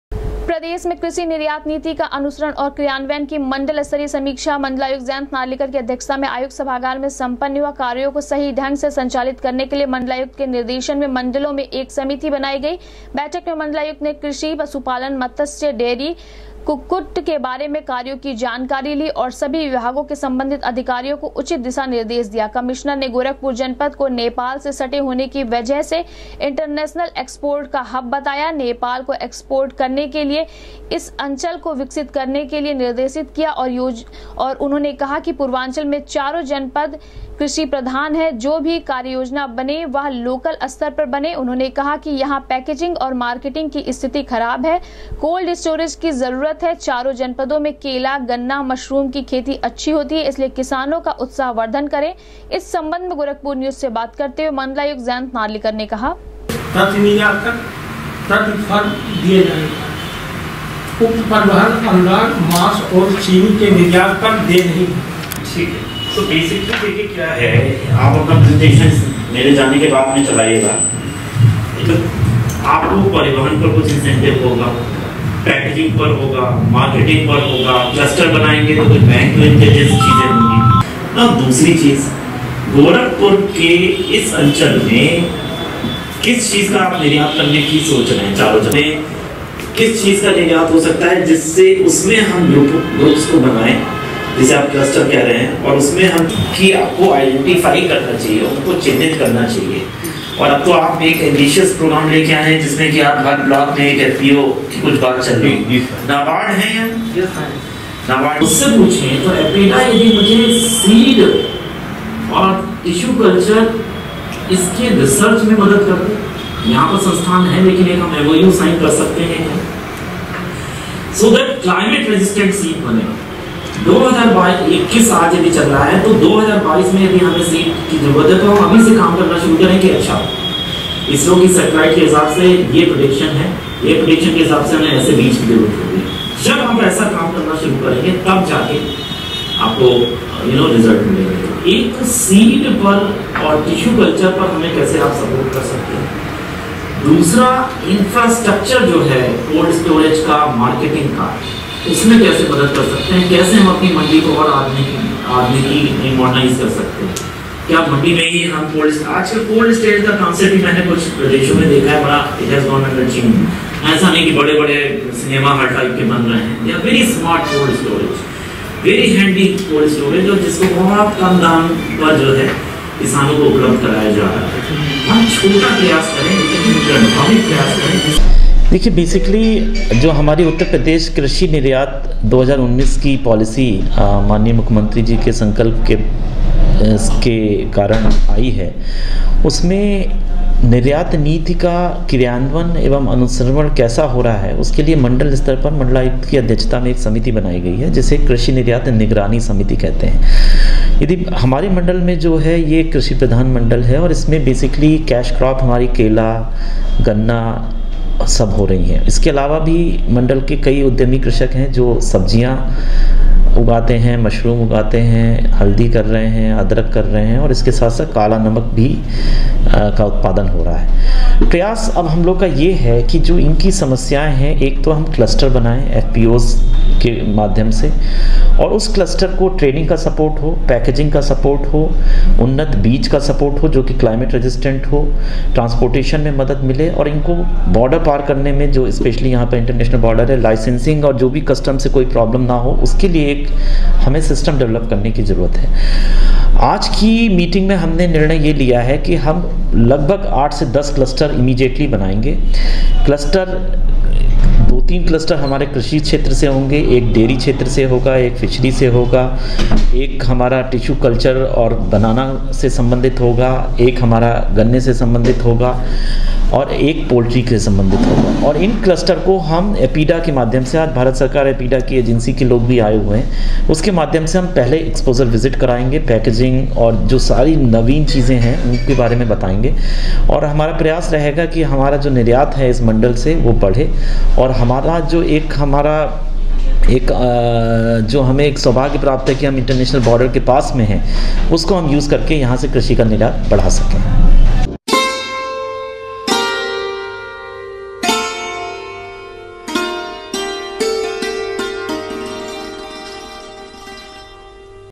मंडलायुक्त की अध्यक्षता में सम्पन्न हुआ कृषि विभाग की मण्डलीय समीक्षा बैठक, कृषि निर्यात नीति का
मंडलायुक्त की अध्यक्षता में सम्पन्न हुआ कृषि विभाग की मण्डलीय समीक्षा बैठक, कृषि निर्यात नीति का अनुसरण और क्रियान्वयन का लिया जायजा,दिए आवश्यक दिशा निर्देश